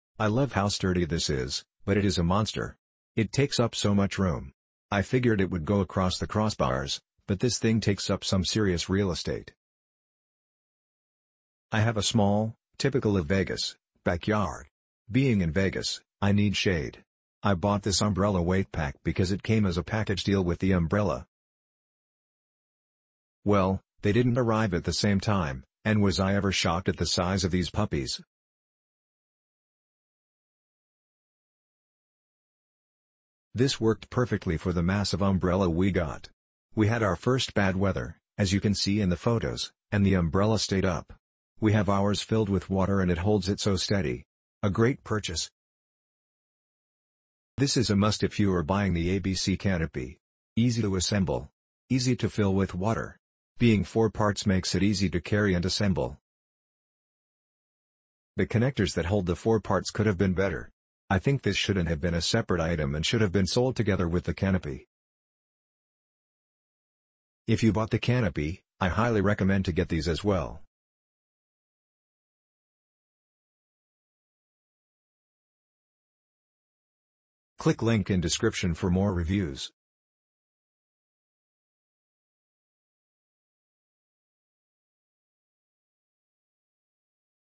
ABCCANOPY Fan Shaped Umbrella Base Fan Style Self Filled Cantilever Offset Umbrella Base Stand Filla
- ABCCANOPY Fan Shaped Umbrella Base/Fan Style Self-Filled Cantilever Offset Umbrella Base Stand Fillable Water Sand, Suitable All Kinds Cross Tiles
 I love how sturdy this is, but it is a MONSTER! It takes up so much room! I figured it would go across the crossbars, but this thing takes up some serious real estate! I have a small (typical of Vegas) backyard. Being in Vegas, I need shade. I bought this umbrella weight pack because it came as a package deal with the umbrella. Well, they didn’t arrive at the same time, and was I ever SHOCKED at the size of these puppies! This worked perfectly for the massive umbrella we got. We had our first bad weather (